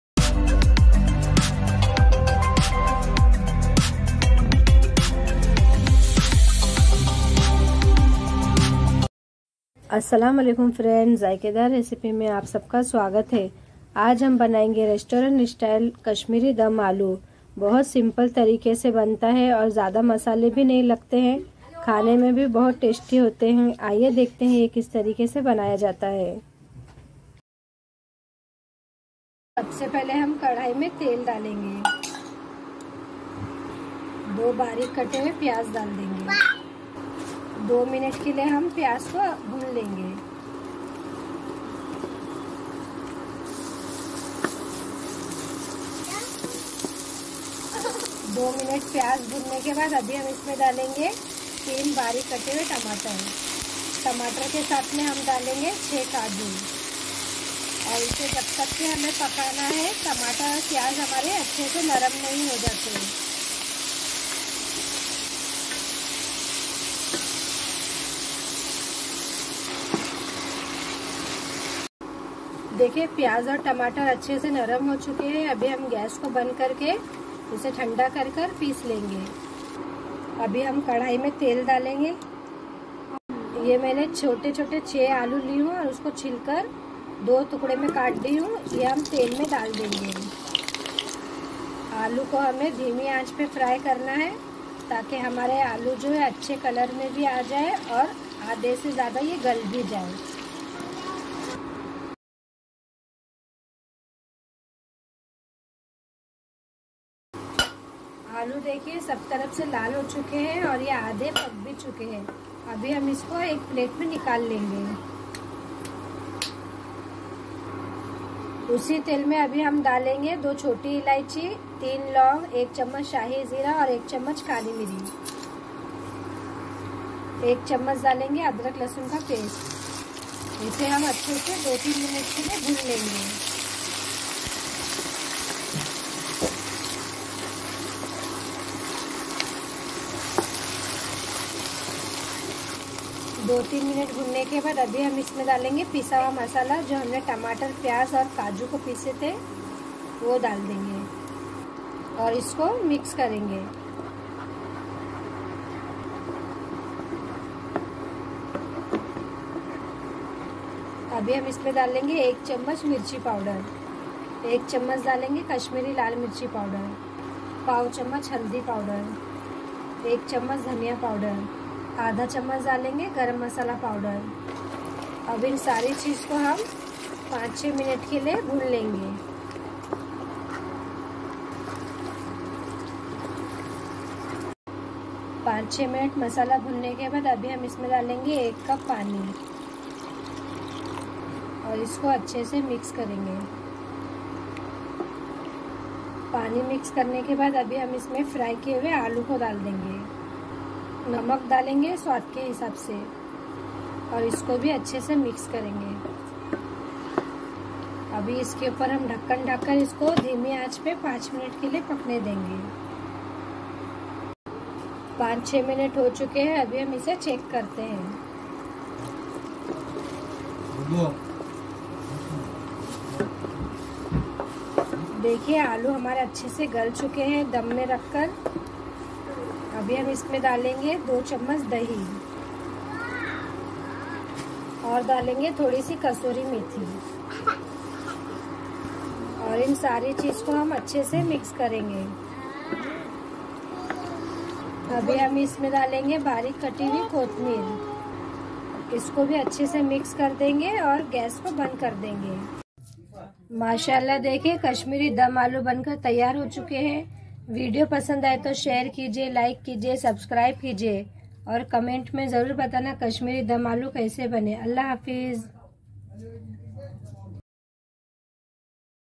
kashmiri dam aloo recipe...ghar me veg khane ka mann kare to banaye dam aloo..simple recipe 😋
ingredients..

1.potato 6
2.onion 2
3.tomato 3
4.kaju 6
5.oil
6.ginger and garlic paste 1spoon
7.elaichi
8.loungh
9.shahi jeera 1spoon
10.black pepar
11.red Chilli powder 1spoon
12.kashmiri powder 1spoon
13.haldi powder 1/2 spoon
14.garam masala 1/2 spoon
15.dhaniya powder 1spoon
16.salt
17.dahi 2spoon
18.kashuri methi
19.coriander leave